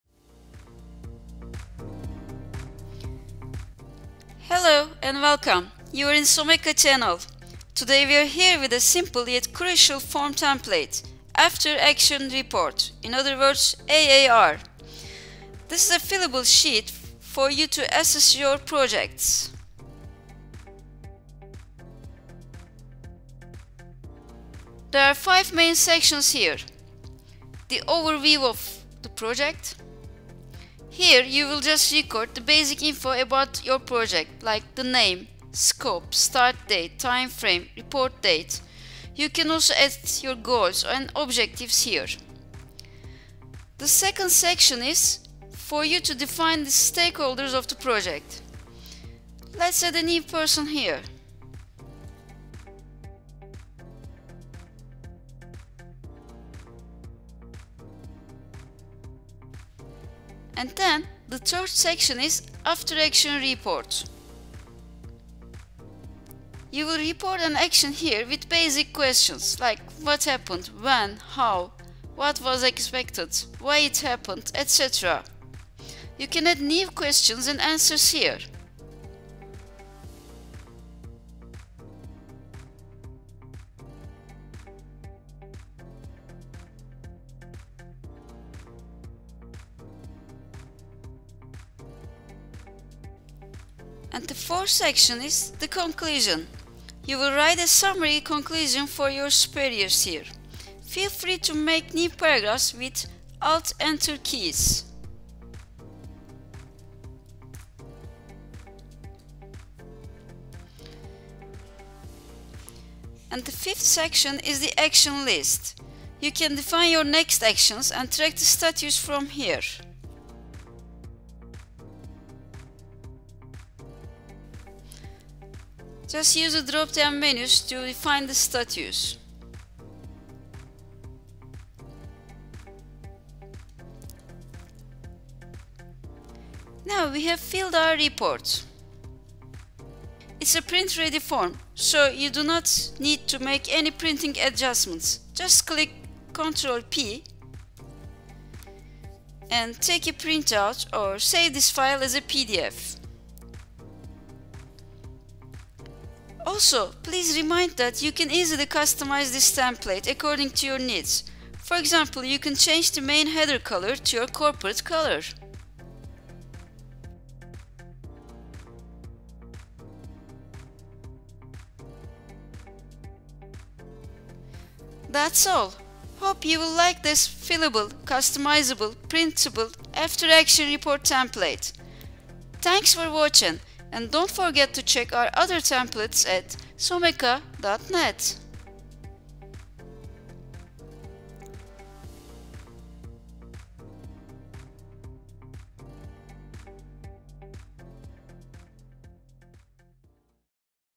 After Action Report | Excel Template | AAR Reporting File
After Action Report template. Monitor, record and present your actions in a professional AAR form. Ready-to-use Excel template for After Action Reporting.

Check this quick tutorial for After Action Report Excel Template to learn more how to use and download the file.

0:00 - Intro
0:30 - Project Overview
0:45 - Project Stakeholders
1:00 - After Action Report
1:35 - Report Summary
1:55 - Action List
2:20 - Printing the report
2:55 - Conclusion

# After Action Report in Excel
# Reporting tool for projects
# Fillable, editable and printable spreadsheet
# Instant download After-Action Review
# Informational notes included
# Ready-to-use template
# Works on both Mac and Windows